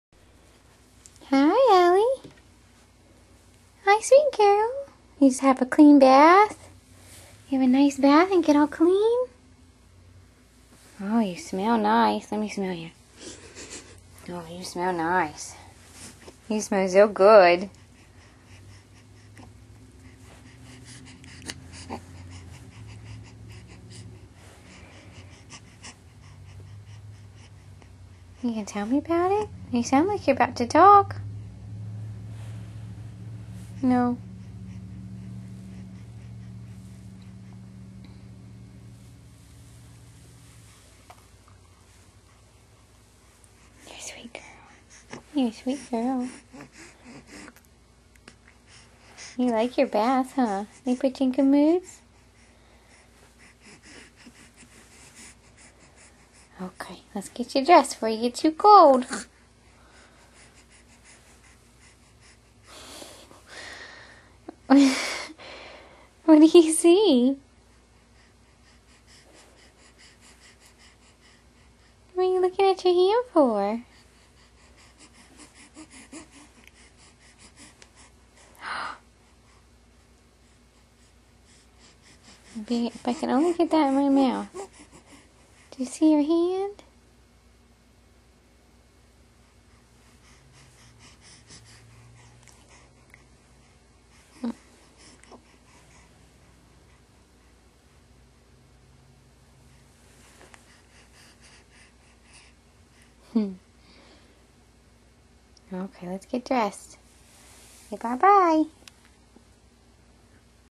Bath
After a bath, Ellie sees her hand.